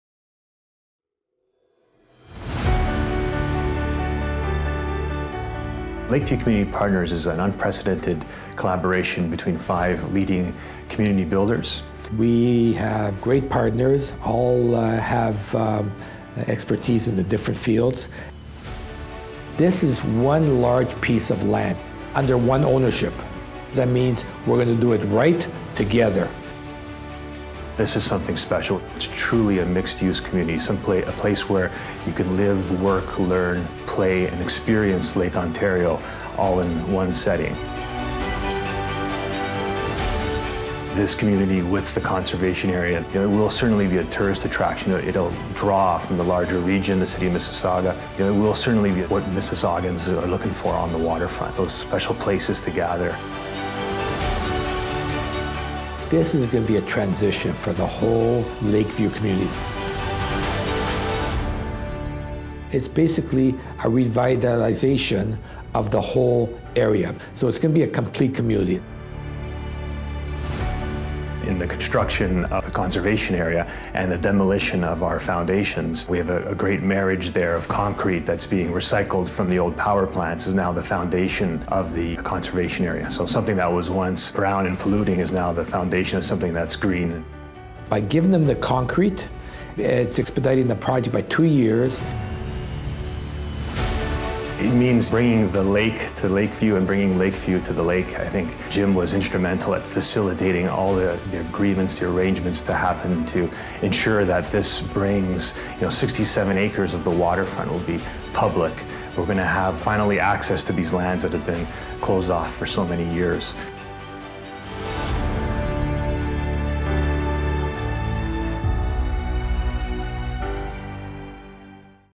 Morphology 2019 Lakeview Community Partners
Credit Valley Conservation, in association with its Presenting Partners, debuts Morphology 2019.

Witness the birth of a new landscape through photography.
Seen through the eyes of 11 creative photo artists, we can explore our relationship and connection with water, landscapes, and our shared responsibility to the Great Lakes.

As Councillor Tovey often repeated, "It's time to bring the lake to Lakeview."